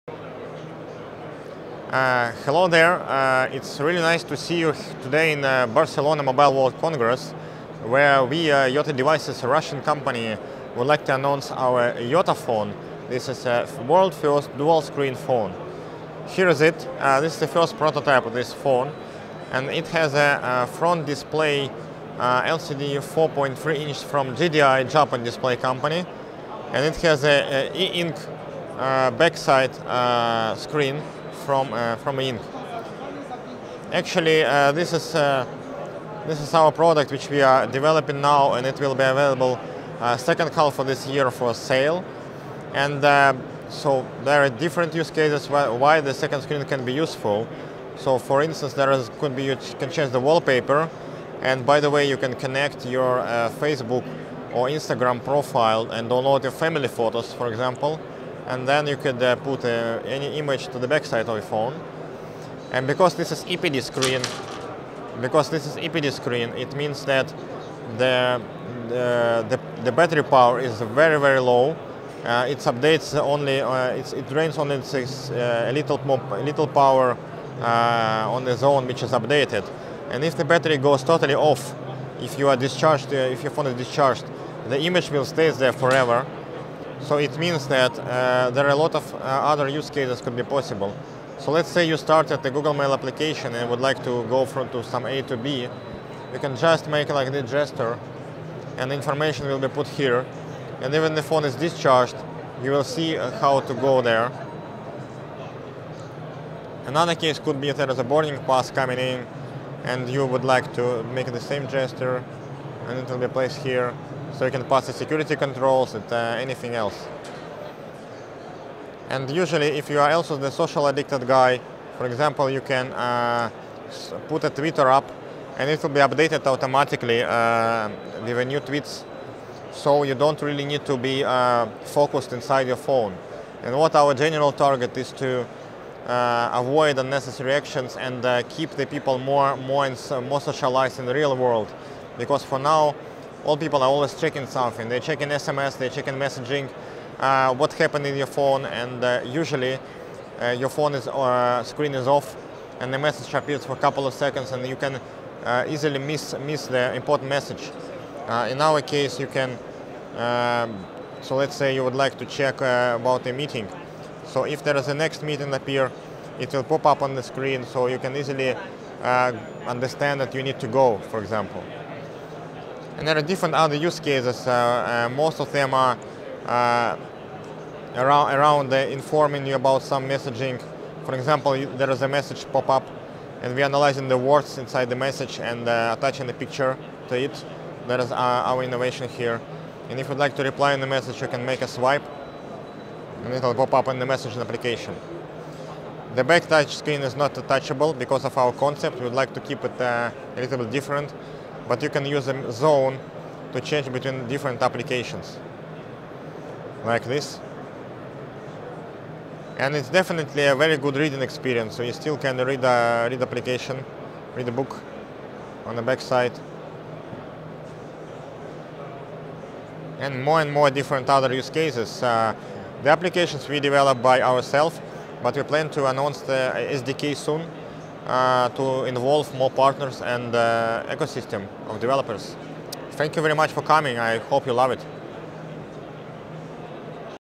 Front: Touchscreen. Back: eInk Display. This Is The Brilliant Yota Phone - Hands-On
The most brillant ideas are those, of which you ask yourself: why did it nobody before? The Yota Phone is one of these: instead of a useless, empty back this Android has an eInk display like that of Amazons Kindle for example. These panels consume power only for changing the image. See this brillant idea in this hands-on video from the Mobile World Congress!